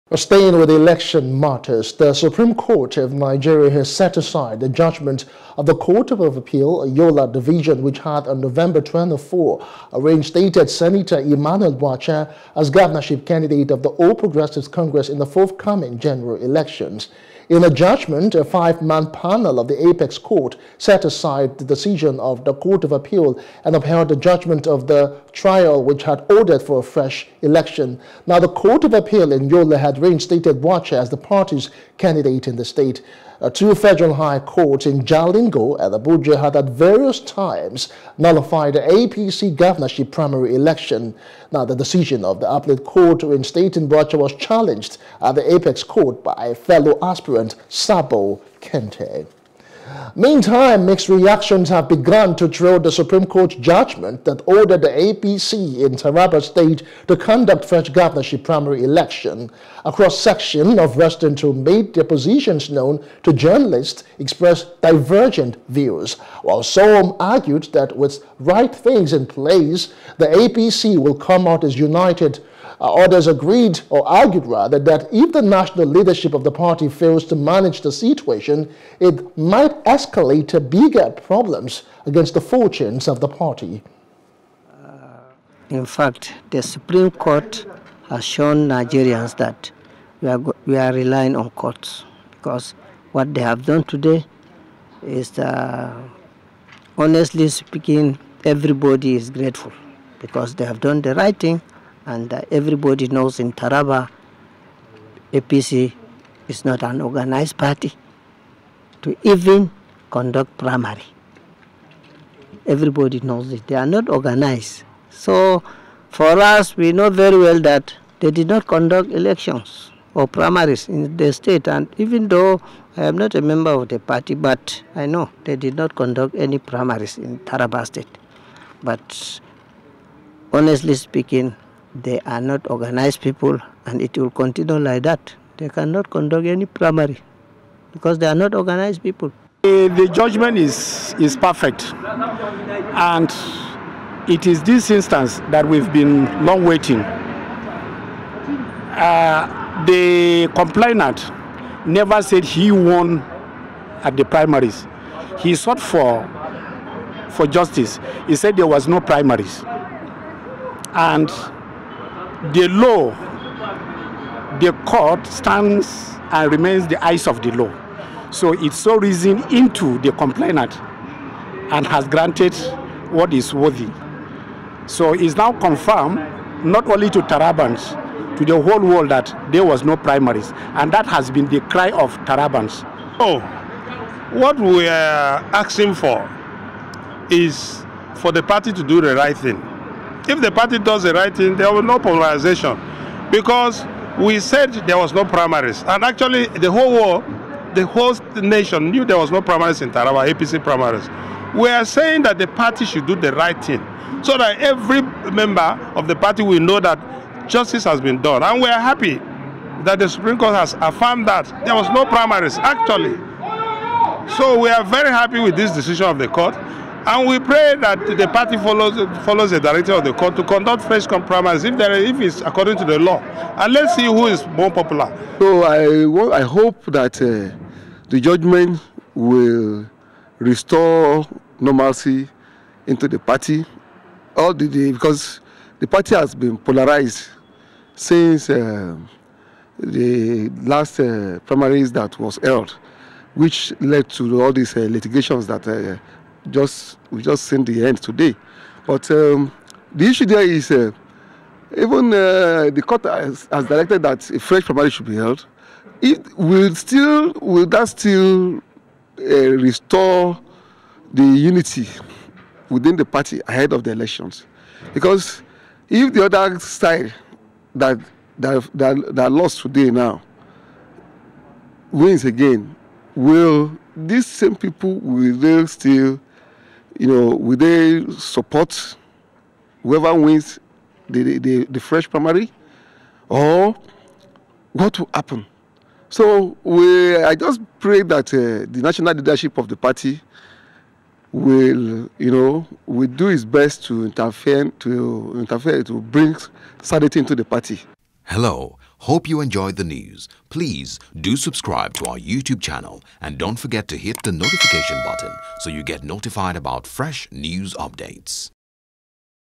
Supreme Court Nullifies Bwacha’s Candidacy .
The Supreme Court of Nigeria has set aside the judgement of the Court of Appeal, Yola Division, which had on November 24, 2022 reinstated Senator Emmanuel Bwacha as governorship candidate of the All Progressives Congress (APC) in the forthcoming general elections.